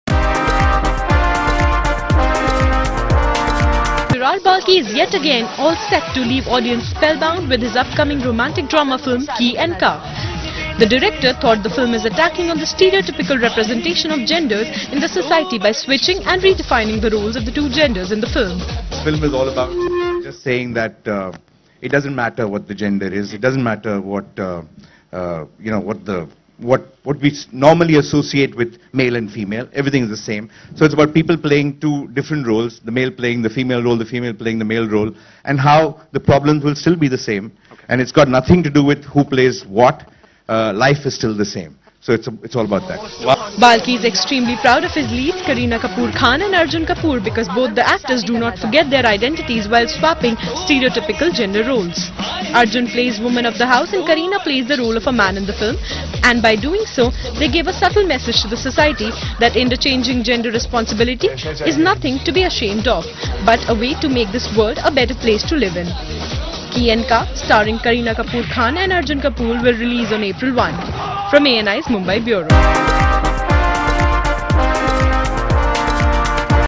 'की एंड का' बदलेगी नजरिया
* 'की एंड का' नए सिरे से स्थापित करेगी हीरो-हीरोइन के रोल
* अर्जुन ने संभाली घर की जिम्मेदारी
* करीना ने बाहर मोर्चा संभाला